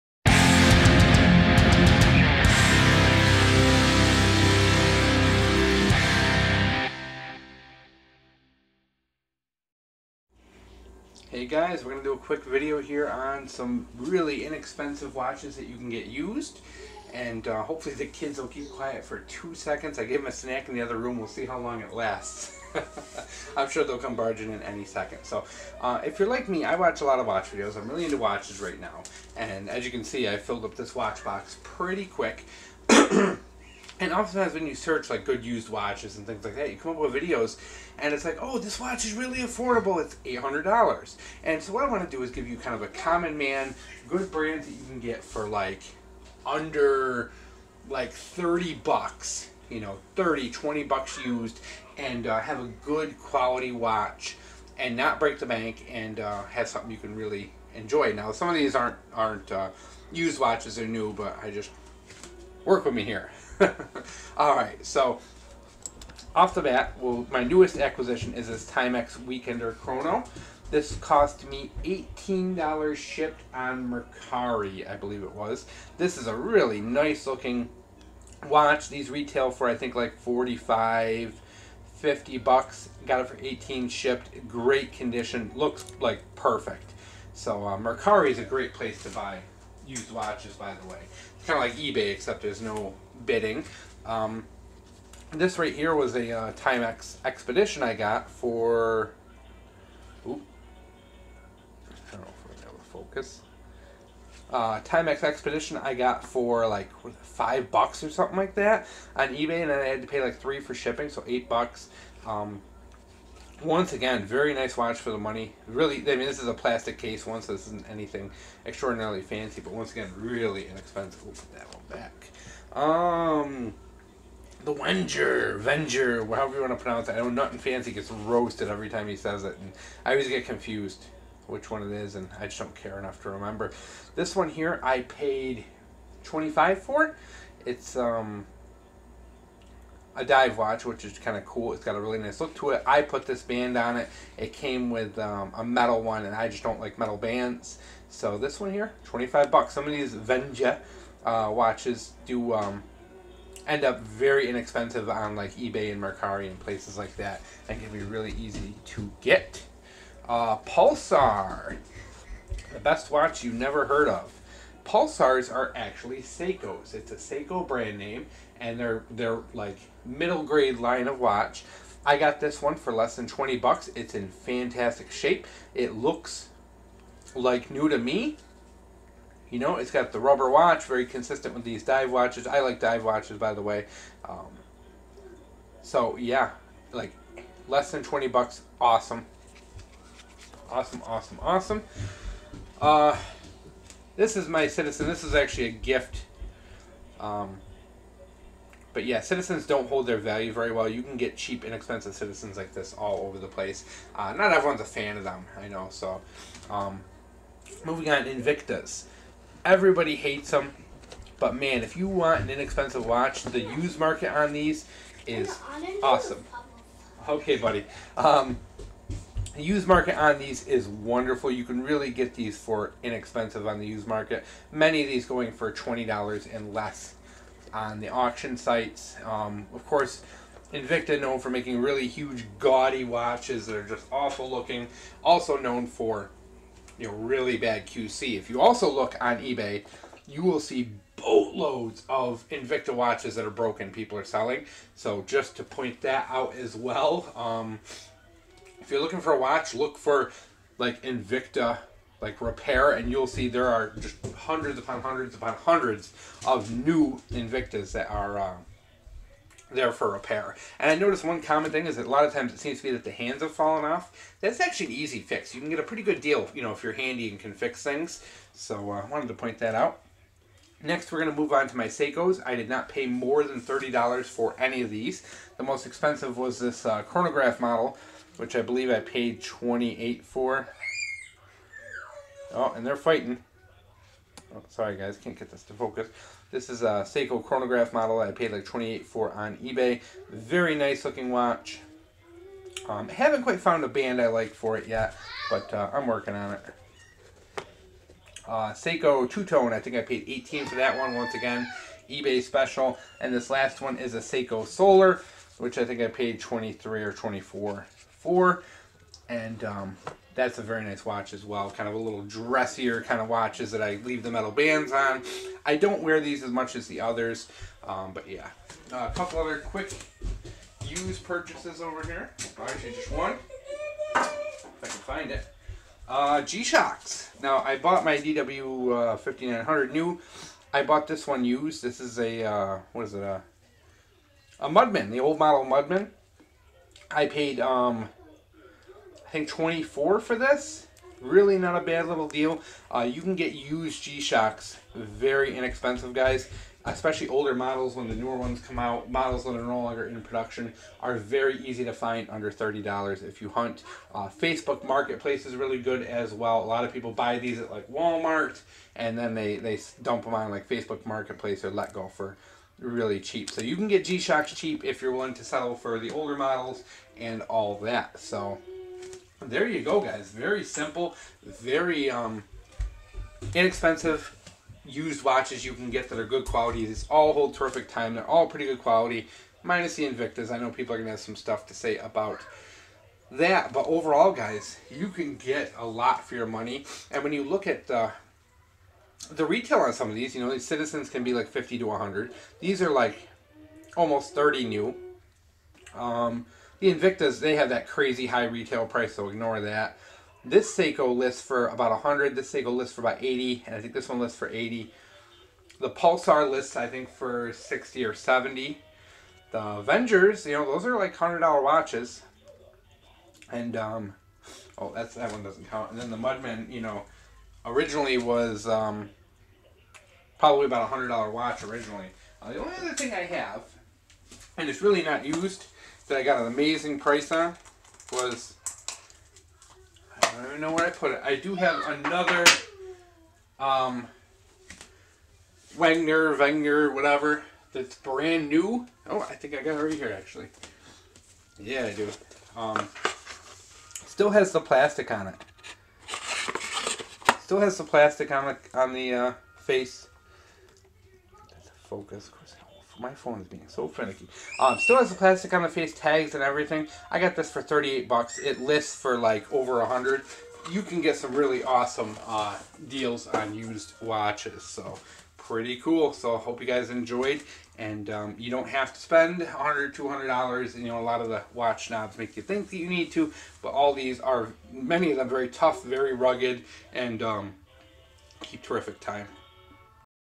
Great used EDC watches that you can buy for cheap
Watches

casio DW5600E
Casio DW9052

Casio W93H illuminator

Casio F91W

Casio F108WH

Casio MDV106

Timex expedition 49900

Invicta 8932